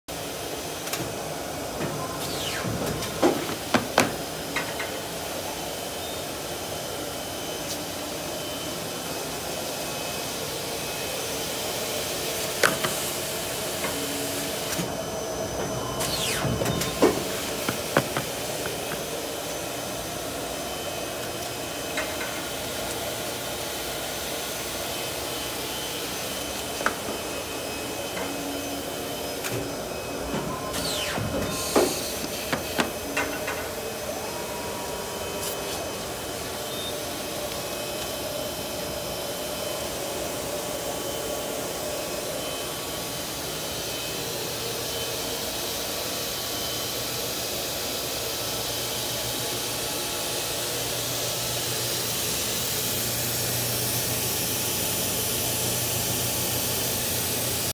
Best Graphics (USA) - Itotec eRC-137DX (54") Cutter Cutting 27# Offset, Lightweight Stock
Our Itotec (Itoh) 45" and 54" dual gear, dual clamp Guillotine Cutters have no problem cutting the toughest of stock, OR the trickiest of lightweight papers. The video above shows a 54" | 137cm Itotec chopping down 4 3/4" lifts of 27# offset stock! Want to know more about our super-duty Cutters, and our industry leading 5-year warranties? and see videos, photos, and full technical spec.